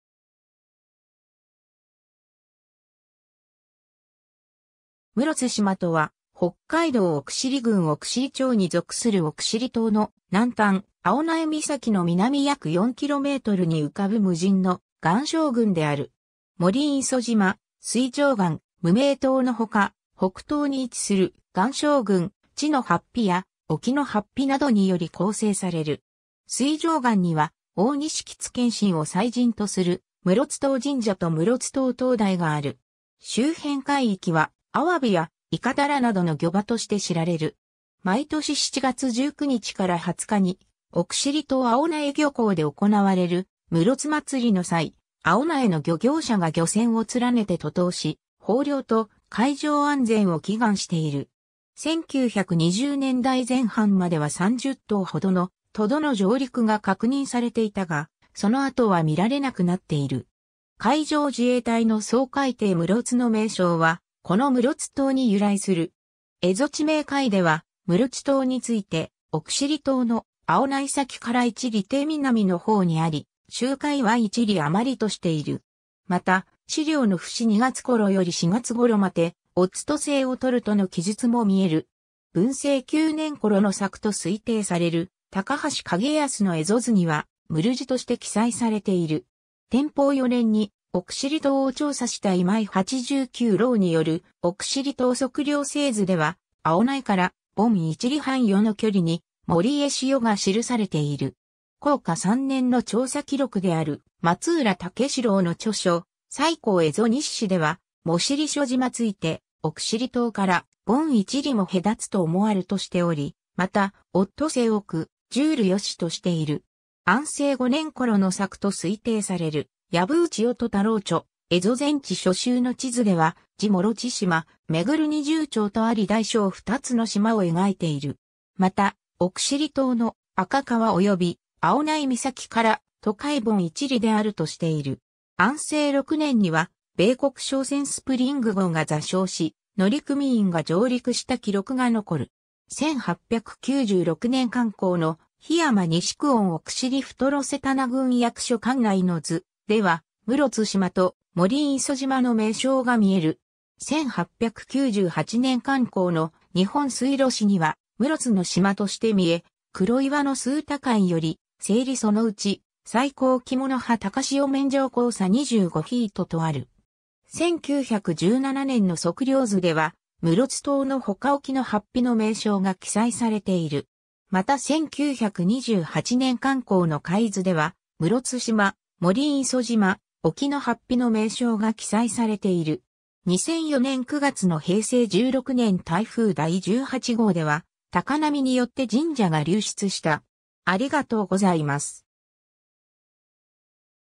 室津島
室津島（むろつじま、むろつしま）とは、北海道奥尻郡奥尻町に属する奥尻島の南端、青苗岬の南約4kmに浮かぶ無人の岩礁群である。
 森磯島（最高標高10m）・水上（みずかみ）岩（最高標高6m）・無名（むめい）島のほか、北東に位置する岩礁群「地（おか）ノハッピ」や「沖ノハッピ」などにより構成される。
水上岩には大錦津見神を祭神とする室津島神社（2006年再建）と室津島灯台（1968年初灯）がある。
 周辺海域はアワビやイカ・タラなどの漁場として知られる。
毎年7月19日から20日に奥尻島青苗漁港で行われる室津祭の際、青苗の漁業者が漁船を連ねて渡島し、豊漁と海上安全を祈願している。
 1920年代前半までは30頭ほどのトドの上陸が確認されていたが、その後は見られなくなっている。
 海上自衛隊の掃海艇むろつの名称は、この室津島に由来する。
 『蝦夷地名解』（北海道立文書館所蔵）では、「ムルチ島」について奥尻島の「アヲナイサキ」から1里程南の方にあり「周回」は1里あまりとしている。
また「私領之節二月頃より四月頃まてヲツトセイをとる」との記述も見える。
 文政9年（1826年）頃の作と推定される高橋景保の蝦夷図には「ムルヂ」として記載されている。
 天保4年（1833年）に奥尻島を調査した今井八九郎による『奥尻島測量製図』では、「アヲナイ」から「凡一里半ヨ」の距離に「モリエシヨ」が記されている。
 弘化3年（1846年）の調査記録である松浦武四郎の著書『再航蝦夷日誌』では、「モシリショ島」（原文ママ）ついて奥尻島から「凡壱里も隔つと思わる」としており、また「膃肭臍多く住るよし」としている。
 安政5年（1858年）頃の作と推定される薮内於菟太郎著『蝦夷全地』所収の地図では、「字モロチシマ」「廻廿丁」とあり大小二つの島を描いている。
また奥尻島の「アカ川」および「アヲナイ岬」から「渡海凡一里」であるとしている。
 安政6年（1859年）には米国商船スプリング号が座礁し、乗組員が上陸した記録が残る。
 1896年刊行の『檜山爾志久遠奥尻太櫓瀬棚郡役所管内之図』では、室津島と森磯島の名称が見える。
 1898年刊行の『日本水路誌』には「室津（モロツ）ノ島」として見え、「黒岩ノ数多塊ヨリ成リ其内最高キモノハ高潮面上高サ二十五呎」とある。
 1917年の測量図では室津島のほか沖ノハッピの名称が記載されている。
また1928年刊行の海図では室津島（Murotu Sima）・森磯島・沖ノハッピの名称が記載されている。
 2004年9月の平成16年台風第18号では高波によって神社が流出した。